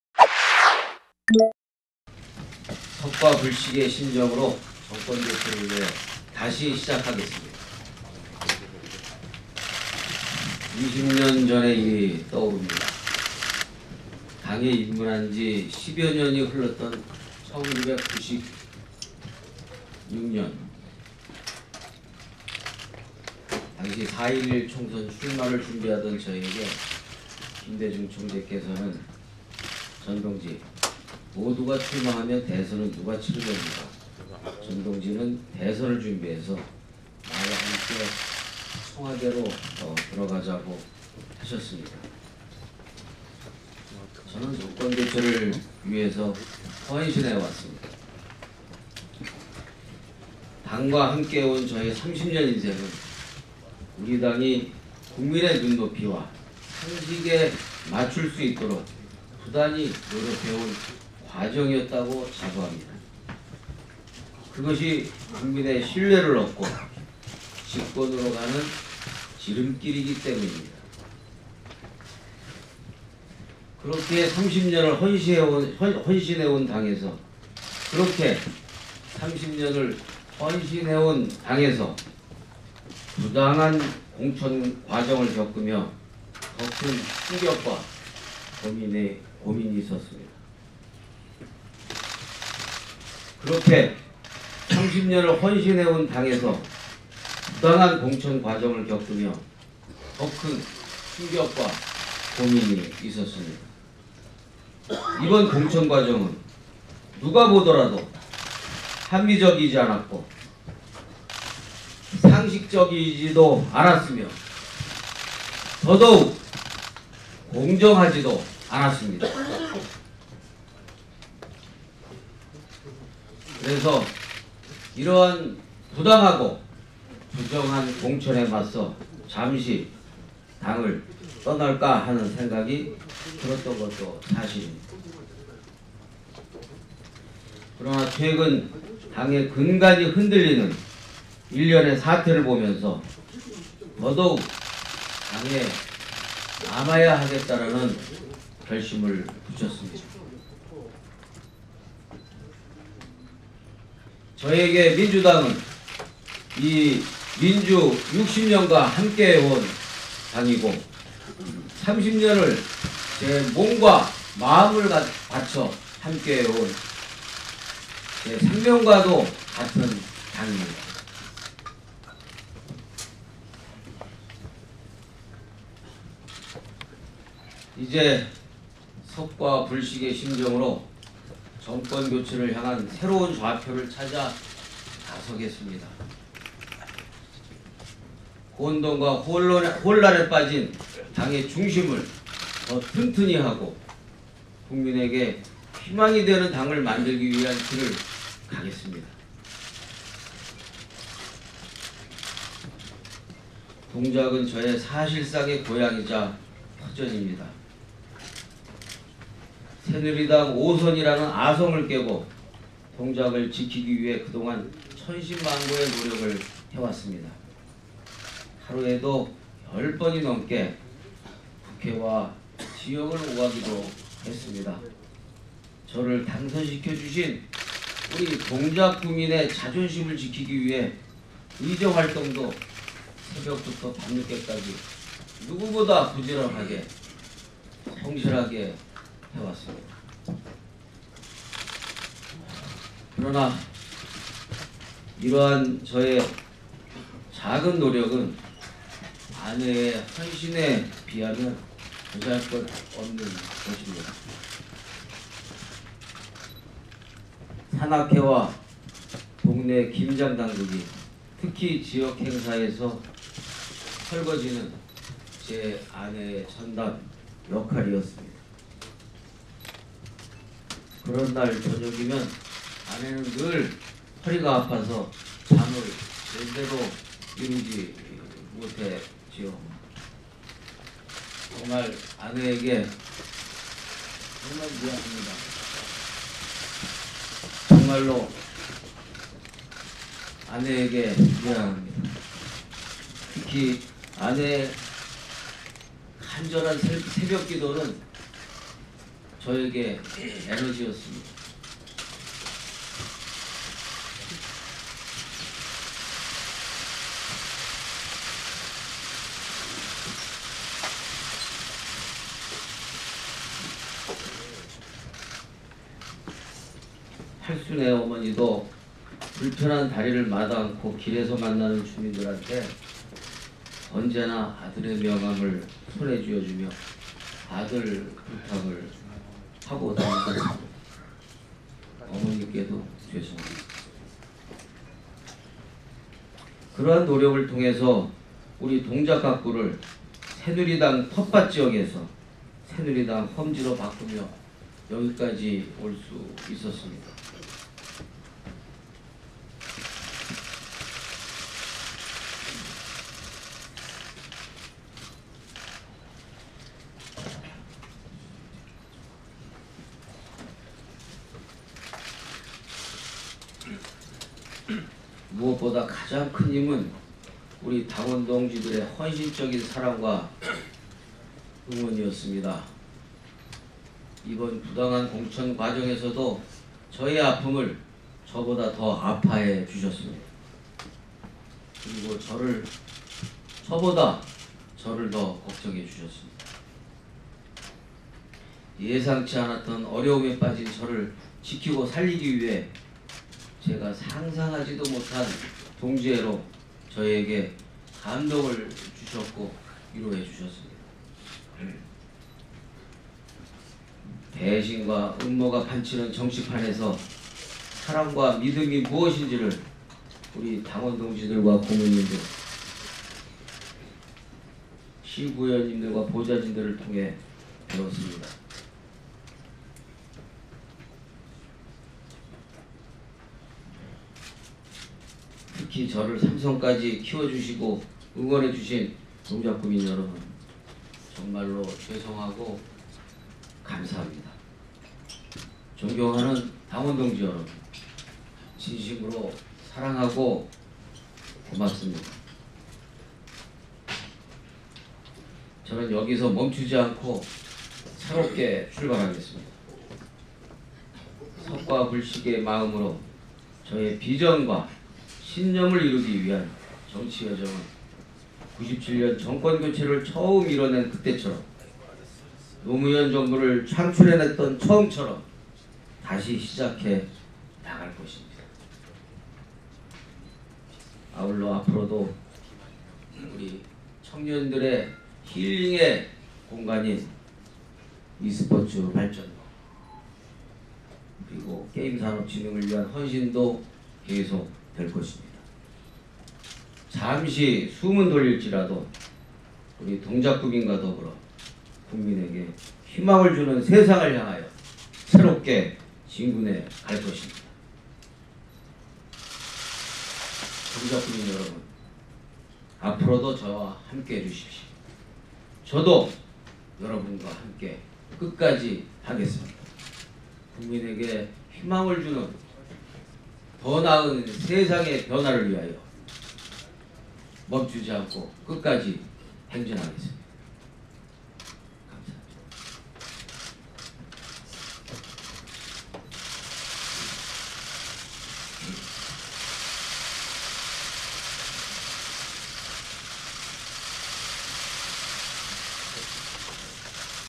[경향신문] 전병헌 “정권교체 위해 당 남아 헌신”
더불어민주당 전병헌 의원(서울 동작갑, 3선)이 당 잔류를 선택하고 불출마를 선언했다. 전 의원은 22일 국회 정론관에서 기자회견을 열고 “‘석과불식’의 심정으로 정권교체를 향한 새로운 좌표를 찾아나서겠다” 고 말했다. 회견장에는 부인과 지지자들이 함께 했다.